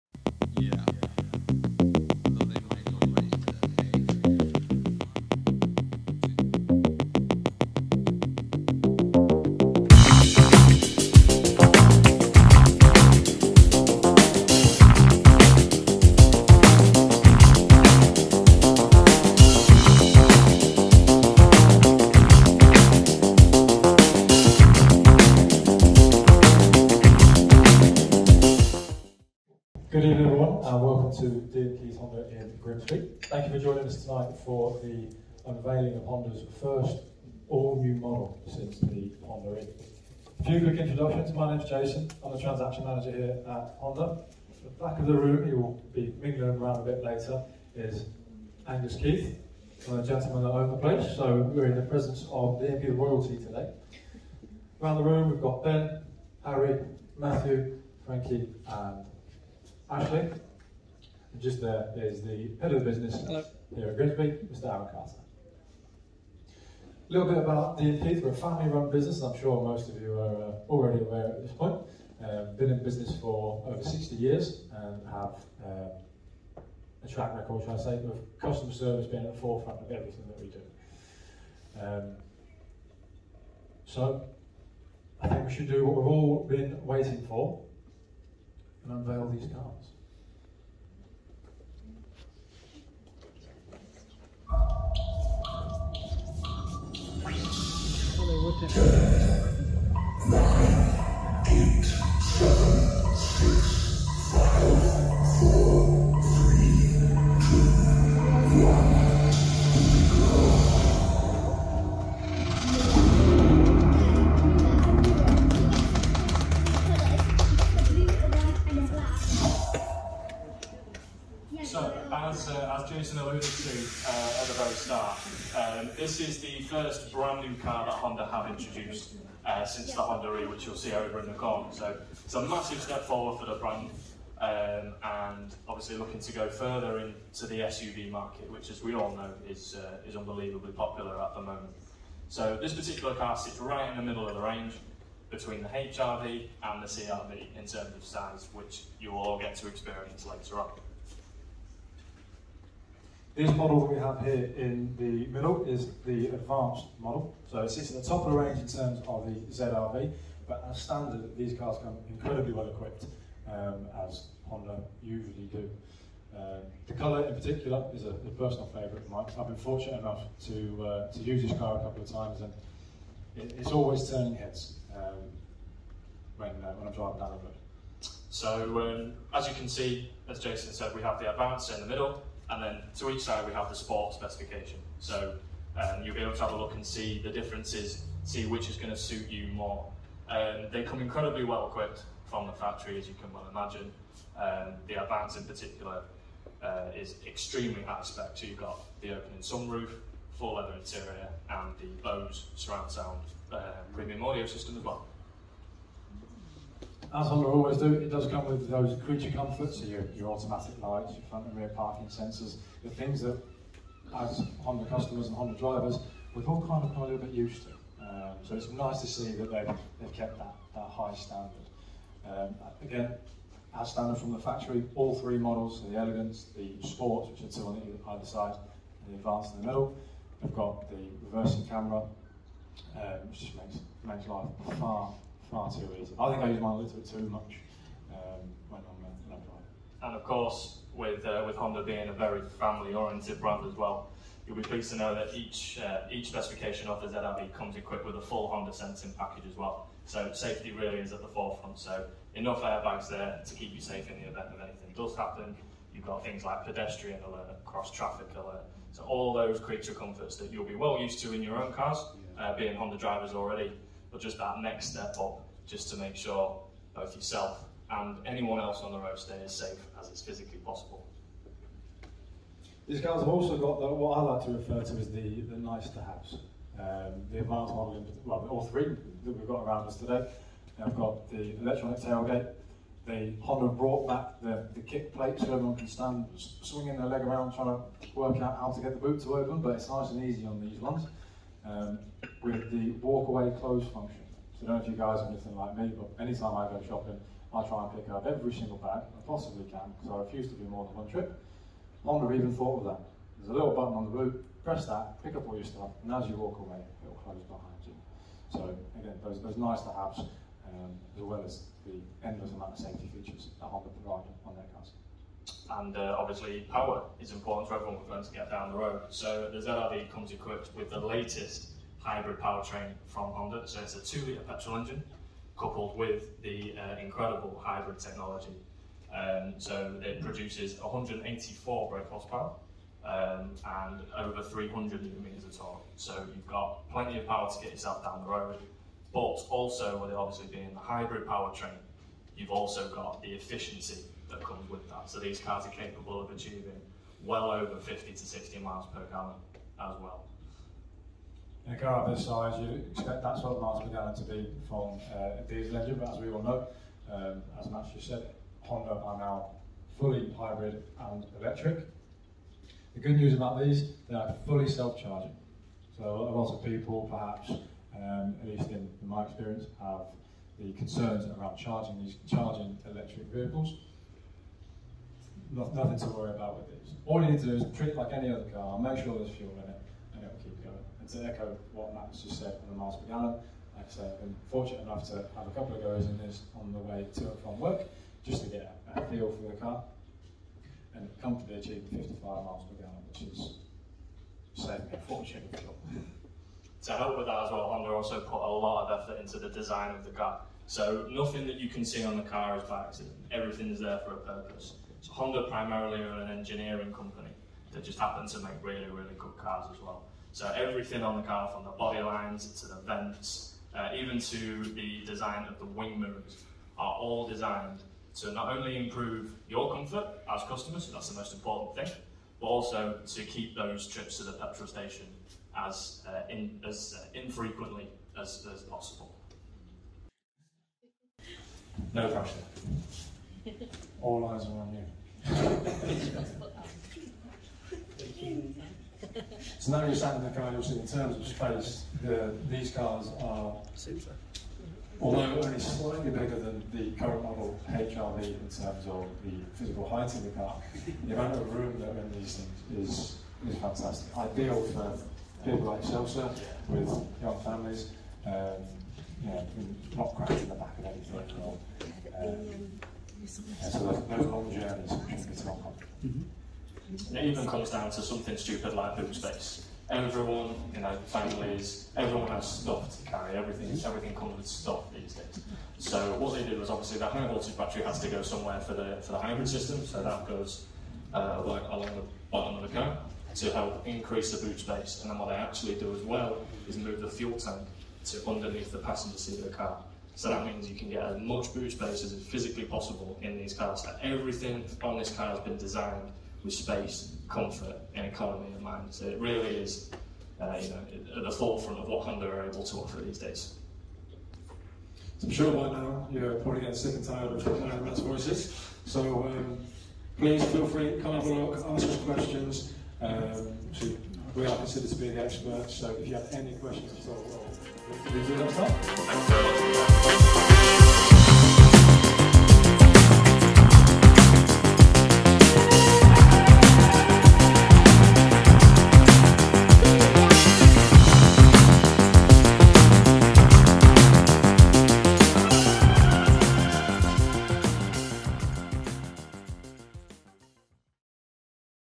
The All-New Honda ZRV: Unveiling the Future of SUVs | 2023 Launch Event and First Look
In this exclusive launch event video, you'll have a front-row seat to witness the cutting-edge features, sleek design, and exceptional craftsmanship that make the Honda ZR-V a true game-changer in the automotive world. Our expert team at D. M. Keith Honda Grimsby will provide an in-depth walkthrough, highlighting all the key elements that set the ZR-V apart from the rest.

Whether you're a die-hard Honda enthusiast or someone looking for a vehicle that delivers on both form and function, this is an event you won't want to miss. Discover the future of driving and experience the Honda ZR-V in all its glory.